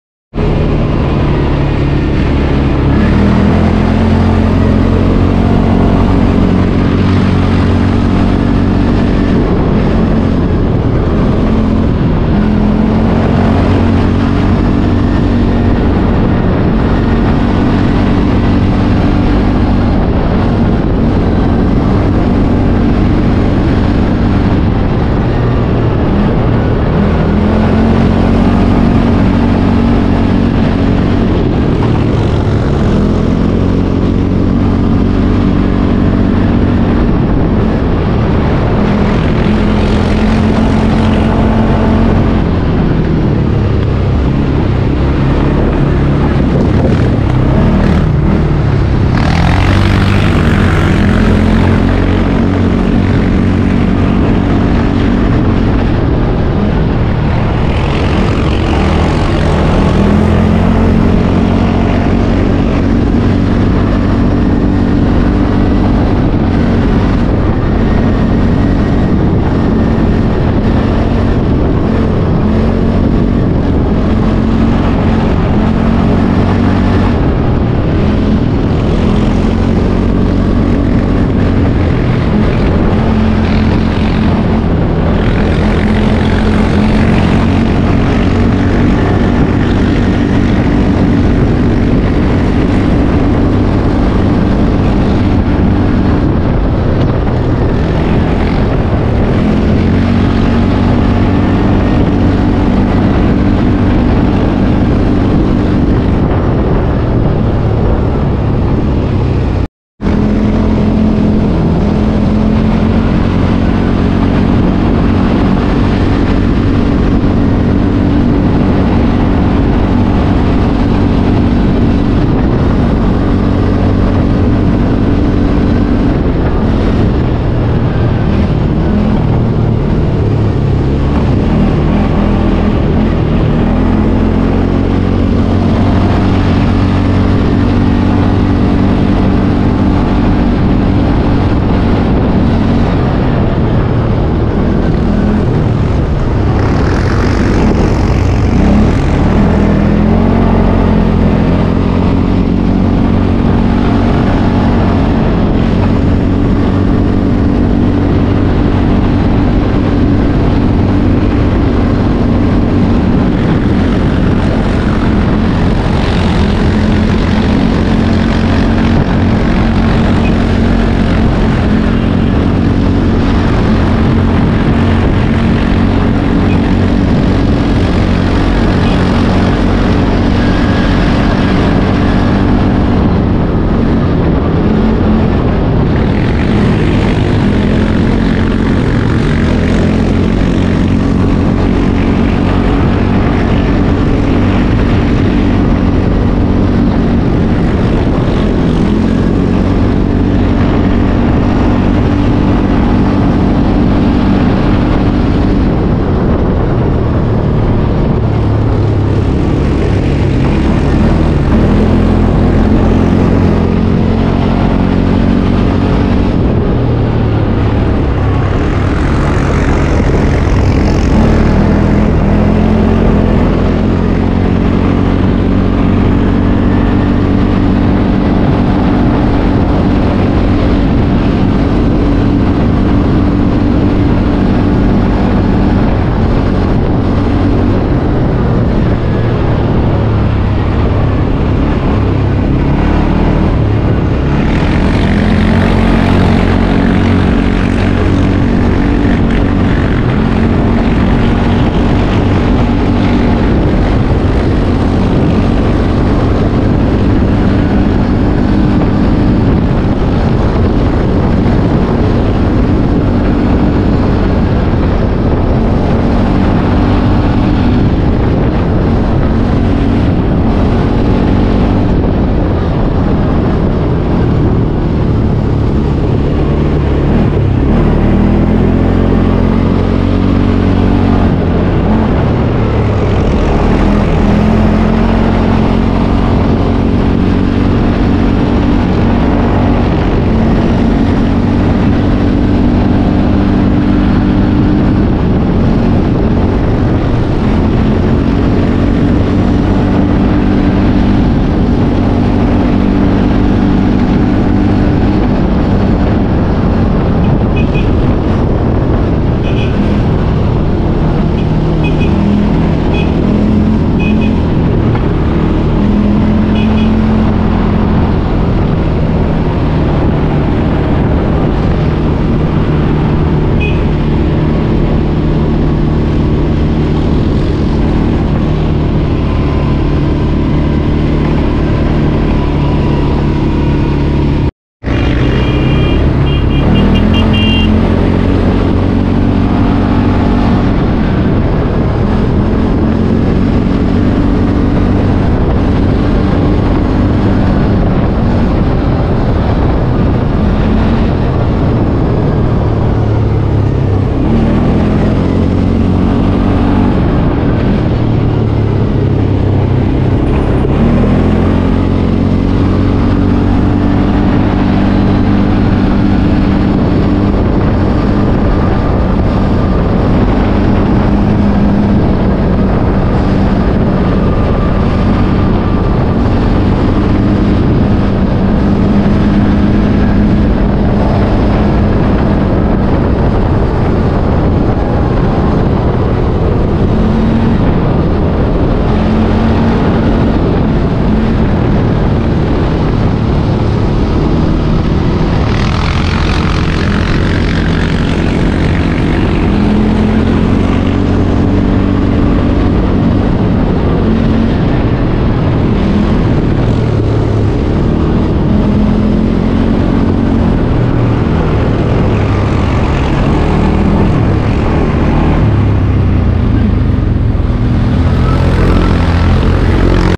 Prokosko Lake Exploration on a CFMoto 550 Quad - Unforgettable Adventure Awaits
Get ready for an adrenaline-packed adventure as we take the CFMoto 600 to its limits on the trail. With its powerful engine and smooth handling, this quad is perfect for high-speed thrills. Join us as we push ourselves and the machine to the max, experiencing the rush of wind in our hair and the thrill of the ride. Don't miss out on this heart-pumping quad riding action, exclusively on mychannel!